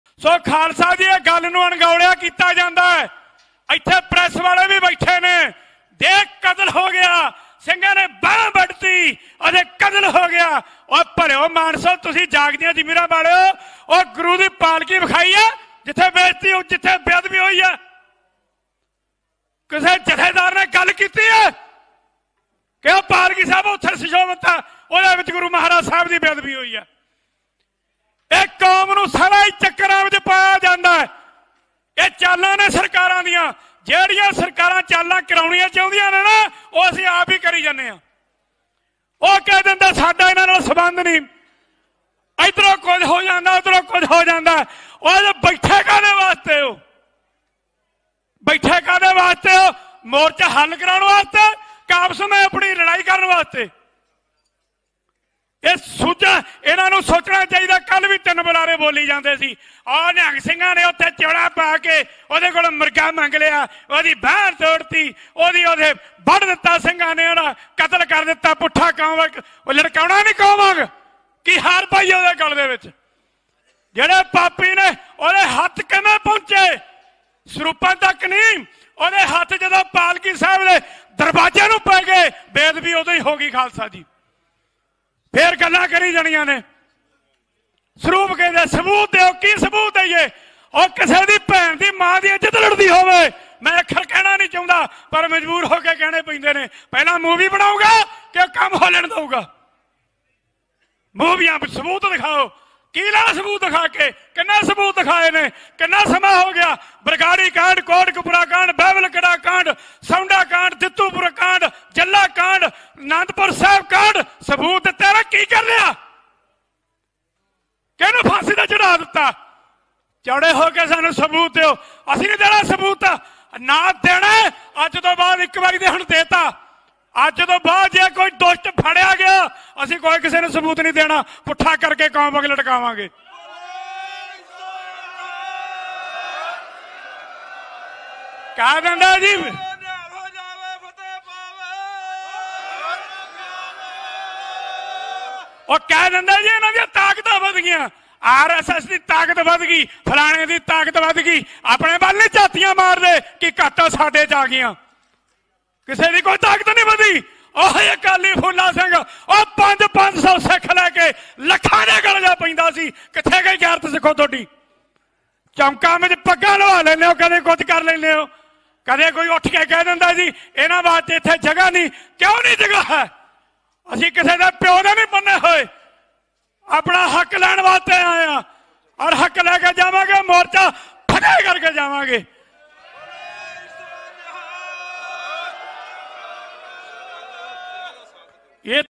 Jathedar Baba Balwinder Singh Ji Nihang Singh on Singhu Border Beadbi
Check us out on other platforms!